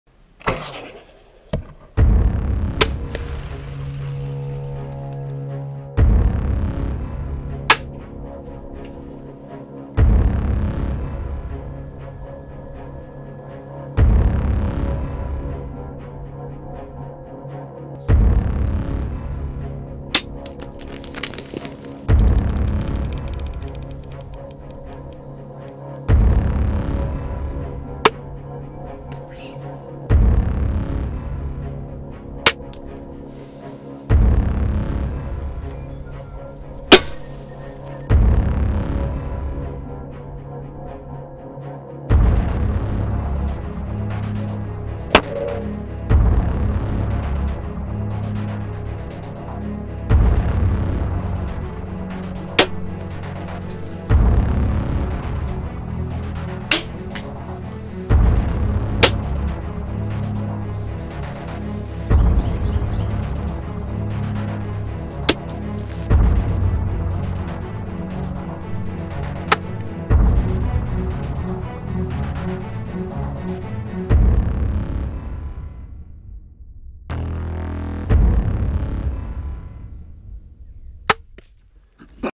Crossbow Hunter Channel Trailer
This is the Crossbow Hunter channel trailer.  It shows several crossbow hunting highlights and hunting locations.  The Channel will also include crossbow hunting and equipment tips and guidelines as well as wild game cooking. There are some awesome and very unique videos coming up!  One of those "one of a kind" videos coming up is a helicopter crash, filmed from inside the helicopter!